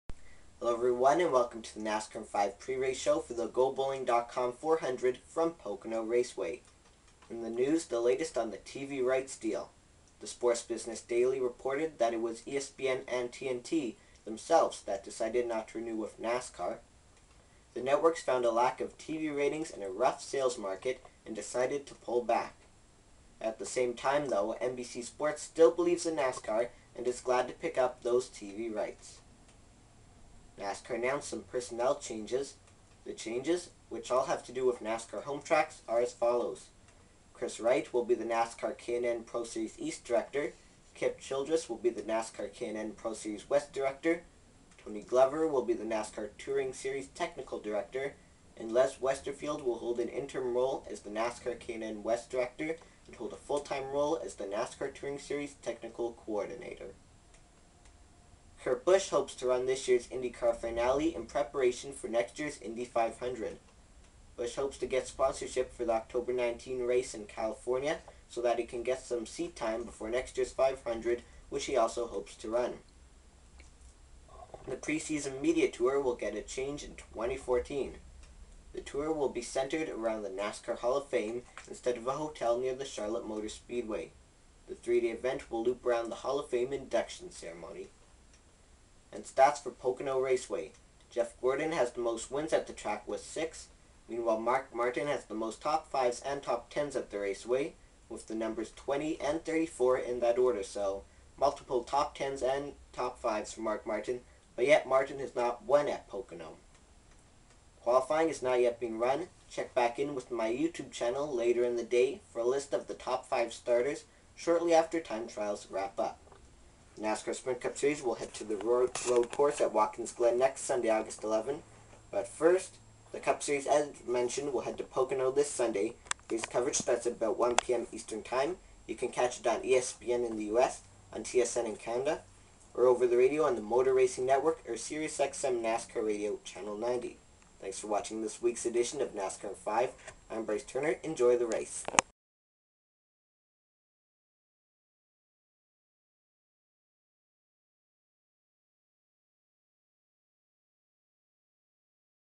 NASCAR in 5 Pre-Race for the Gobowling.com 400 from Pocono Raceway
Getting you set for the NASCAR Sprint Cup race from Pocono with the latest news, schedule, and more!